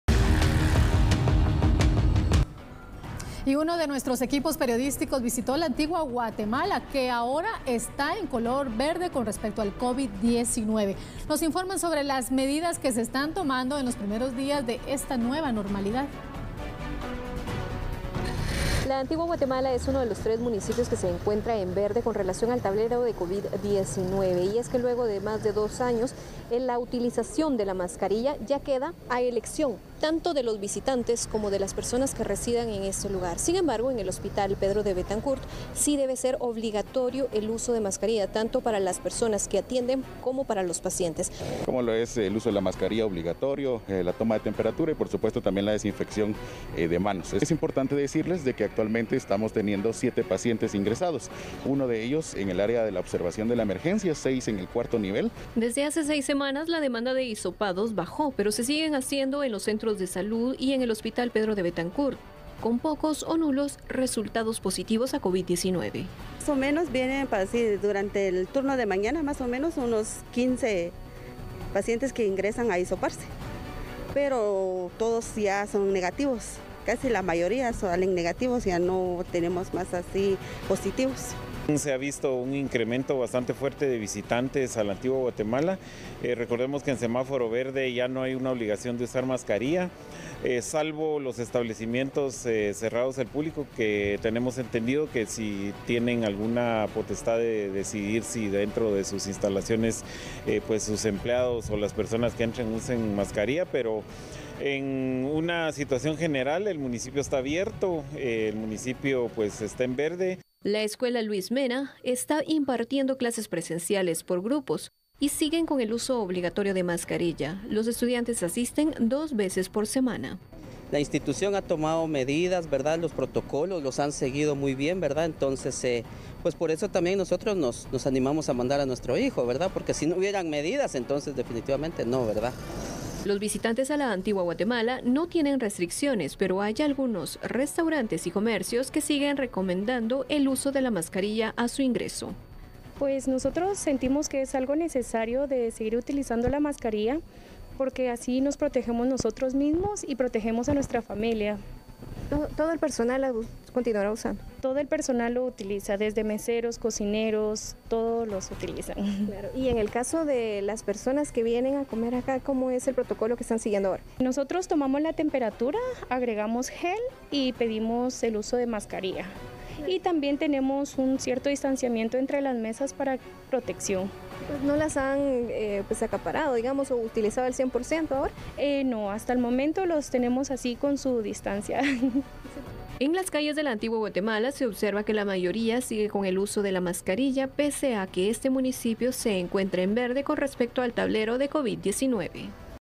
Antigua Guatemala en alerta verde respecto al Covid-19
Después de la aprobación y publicación de las nuevas medidas contra el Covid-19, Antigua Guatemala entró a alerta verde, por lo que se dejarán de usar las mascarillas en espacios abiertos.